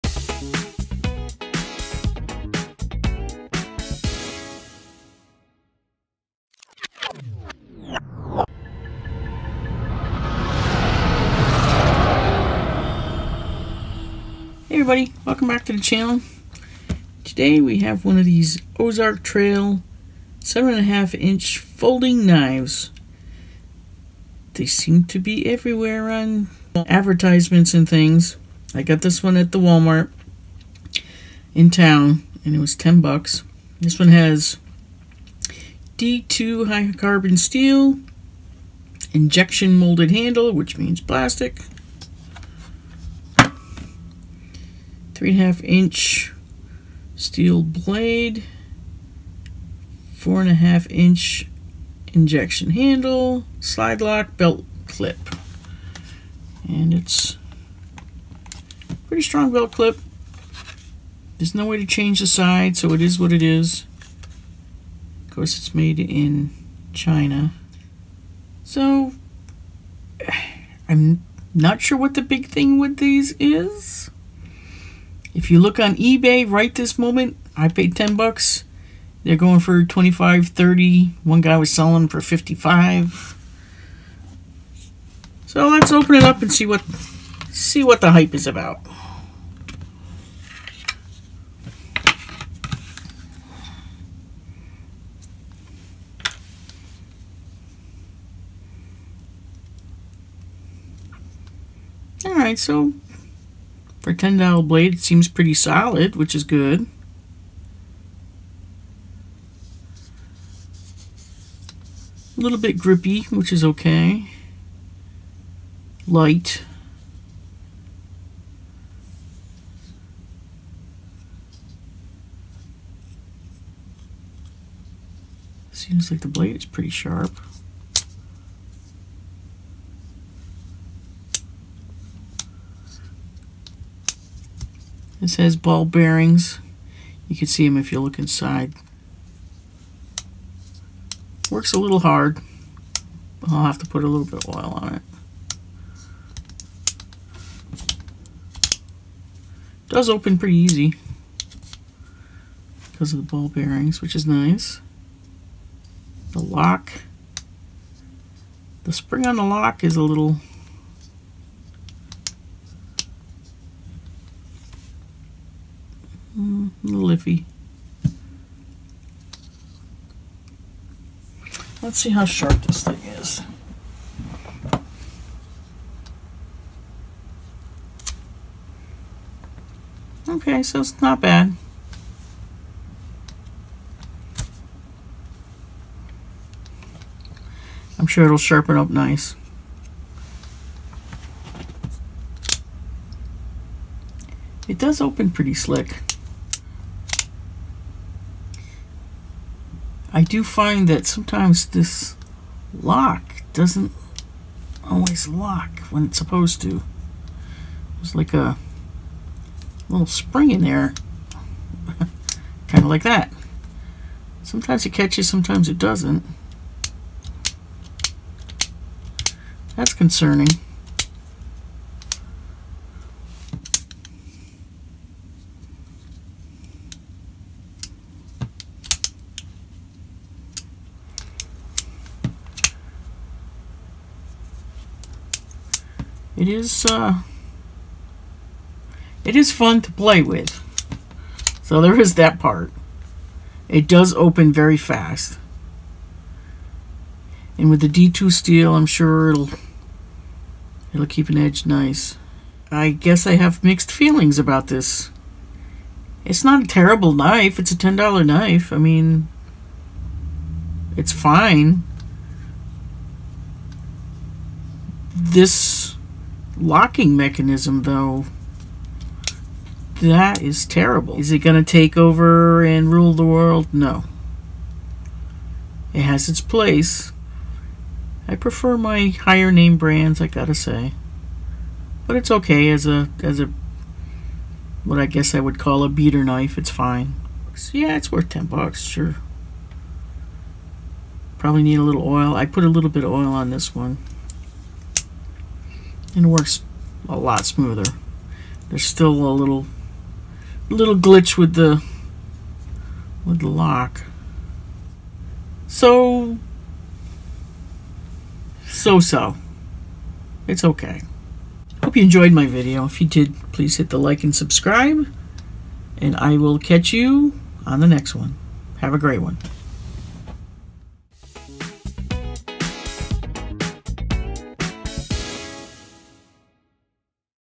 Ozark Trail Folder with D2- Is It Worth the Hype?
Checking out the supposedly amazing Ozark Trail folder
with ball bearings and D2 steel.

Great $10 knife.

Wouldn’t pay much more than that...
But that’s only my opinion.